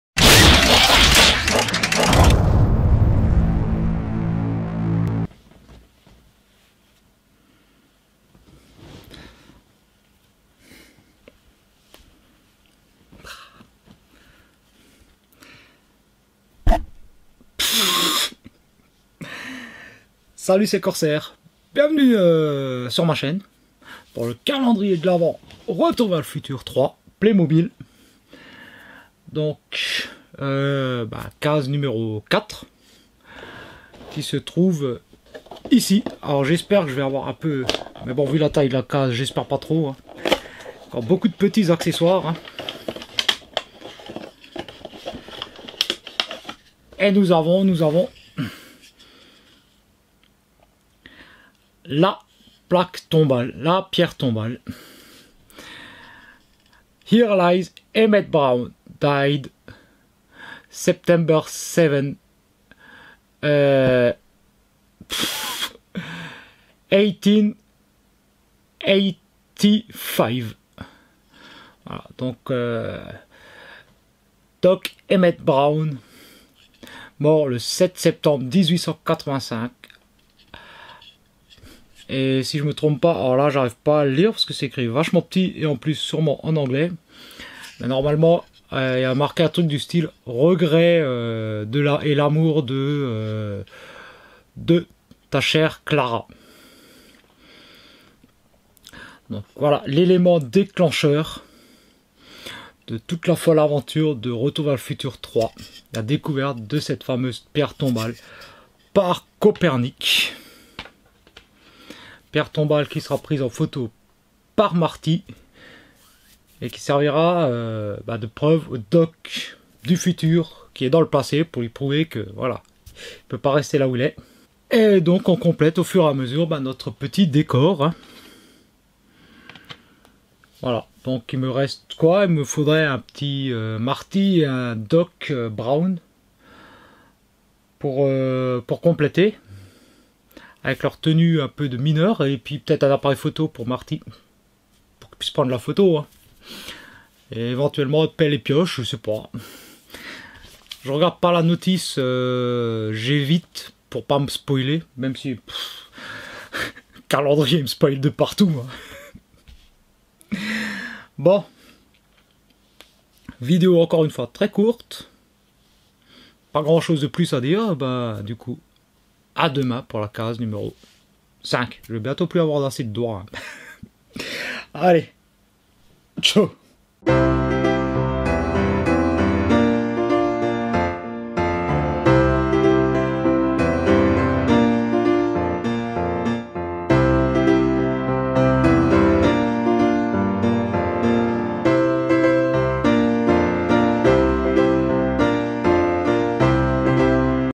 calendrier de l'avent BTTF jour 4
Suivez l'ouverture du calendrier Playmobil Retour vers le futur 3 avec moi. On ouvre une nouvelle case tous les jours a 18h